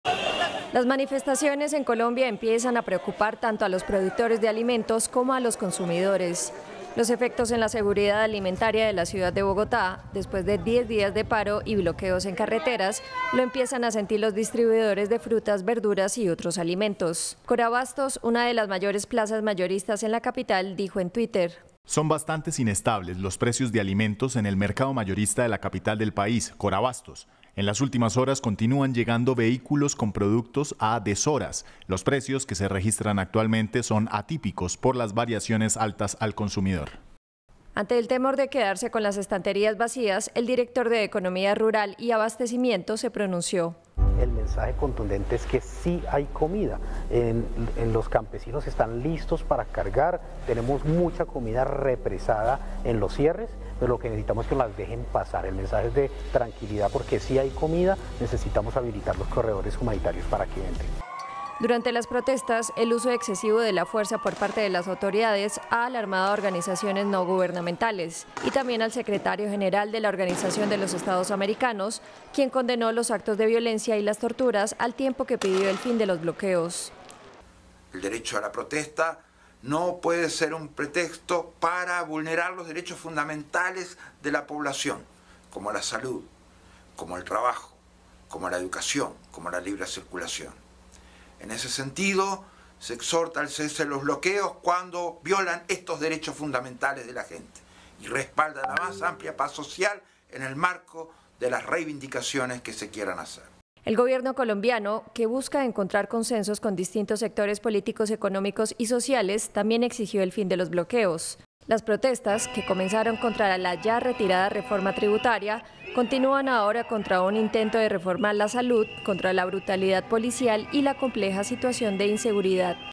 Colombia: Bogotá siente el desabastecimiento de alimentos por bloqueo de vías
En Colombia, las manifestaciones que cumplen 11 días han generado bloqueos en varias vías del país, lo que ha impedido a productores y distribuidores de alimentos llegar a los grandes centros de abastecimiento como sucede en Bogotá, la capital.